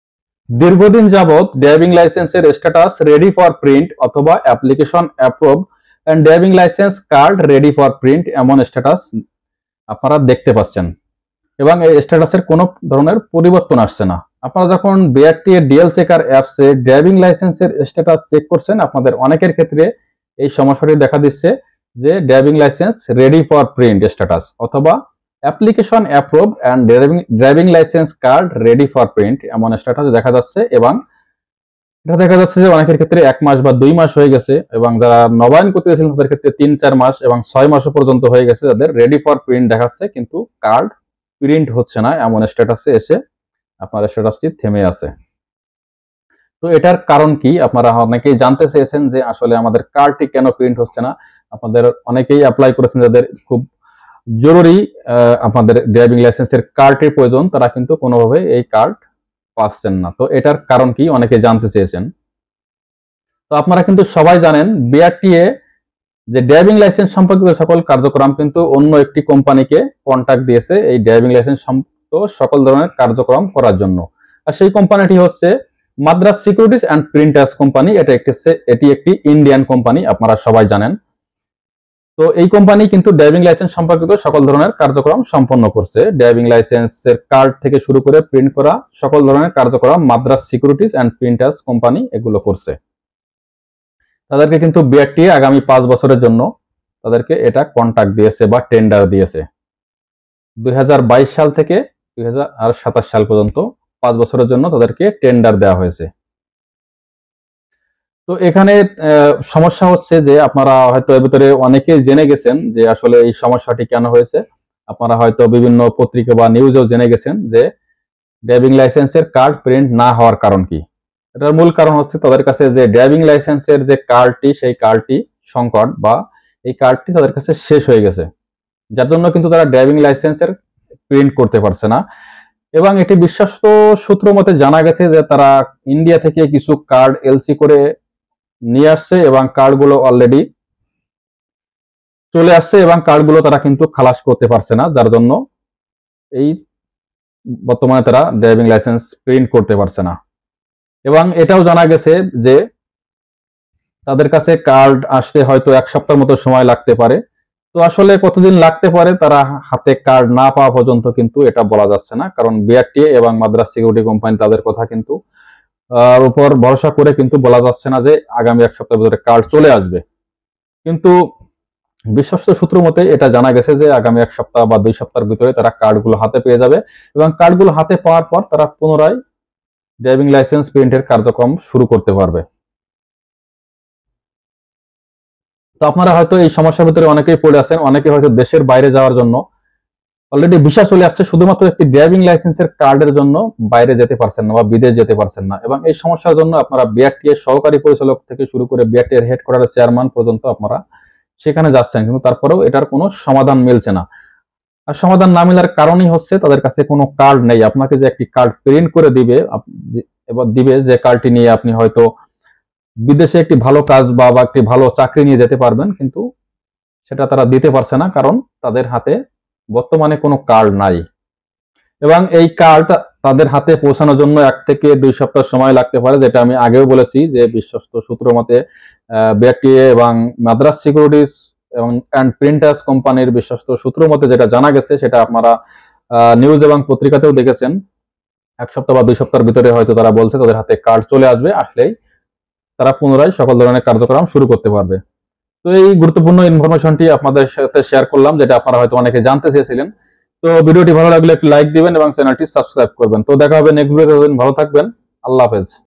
ড্রাইভিং লাইসেন্স কার্ড প্রিন্ট বন্ধ কারন কি? What is the reason for driving license card print stop
ড্রাইভিং লাইসেন্স কার্ড প্রিন্ট বন্ধ থাকার কারন কি | What is the reason for driving license card print stop